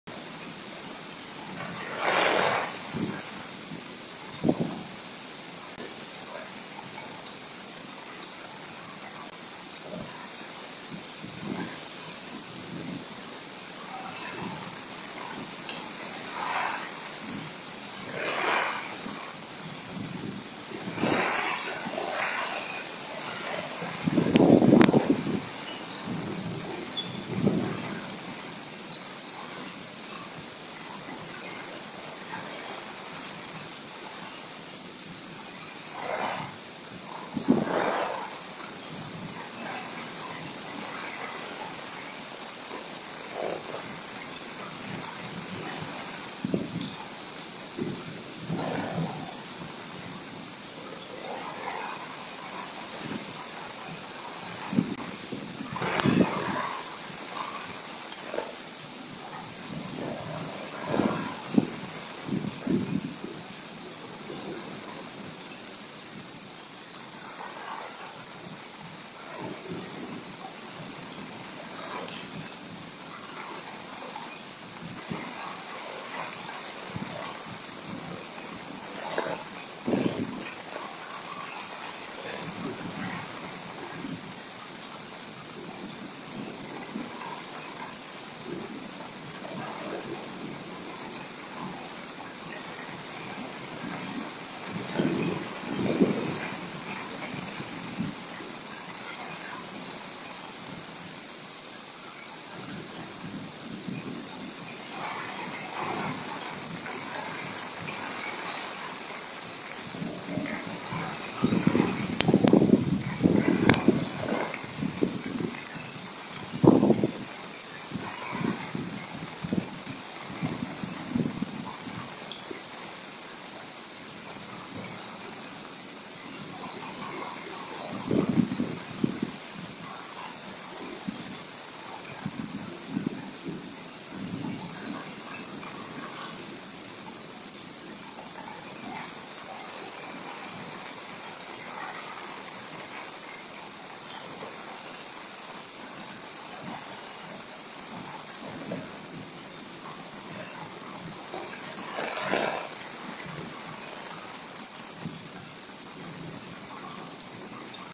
Stormy Summer Night
A storm captured by our ring camera on the deck.  Loud scary summer night.